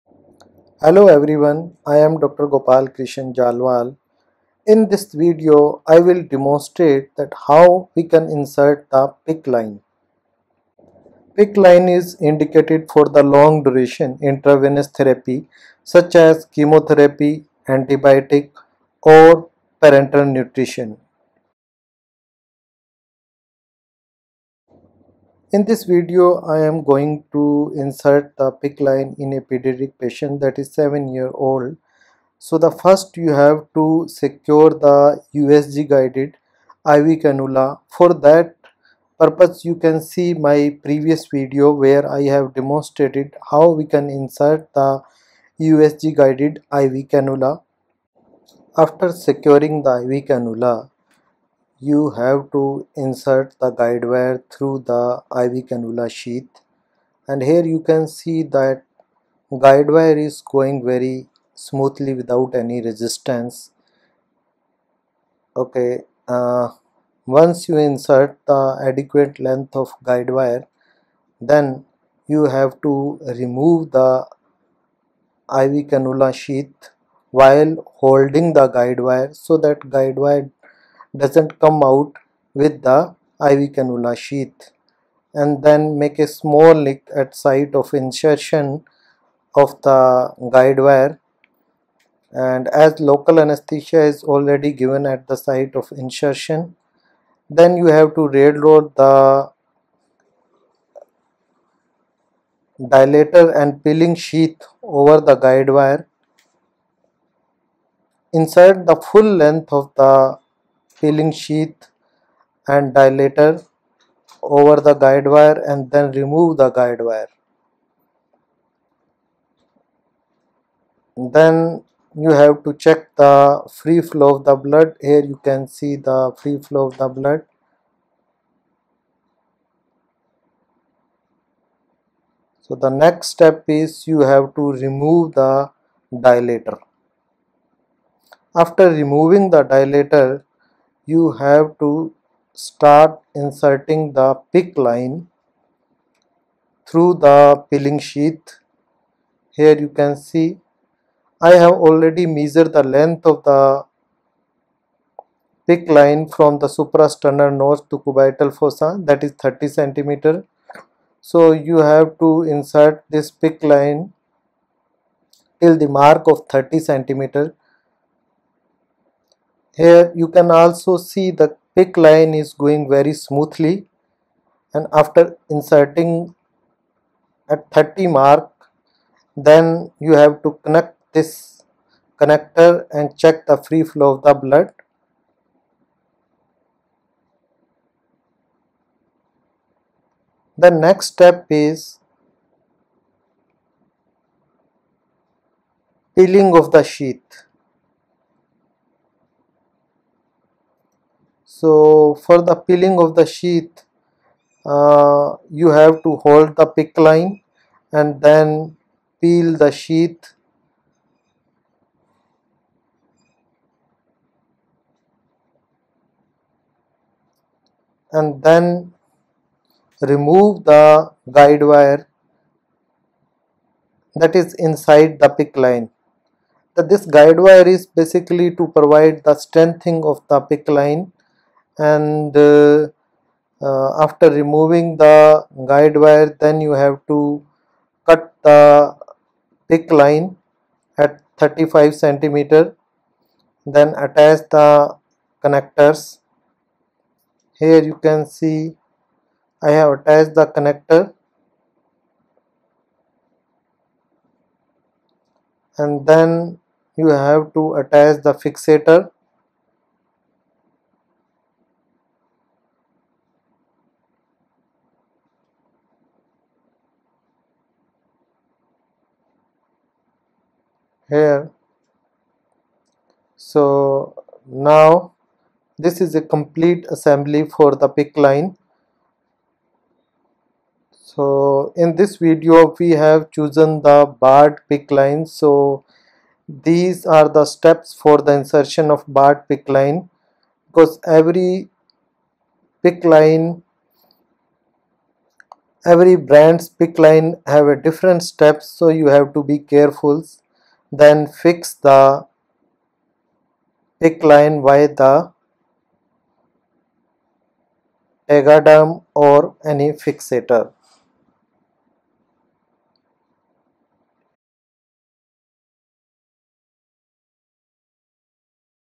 PICC LINE INSERTION
This video is about how to insert PICC line ( peripheral inserted central catheter)